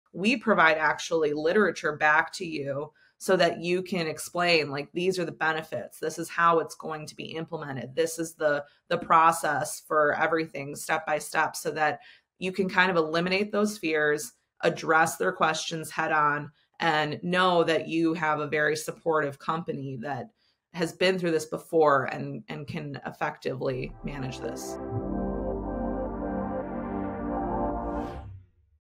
Need guidance with change when upgrading tech. UptimeHealth can show you how they do it for clients
Taylor discusses corporate changes in the tech used, and how you can upgrade smoothly when selling to a client.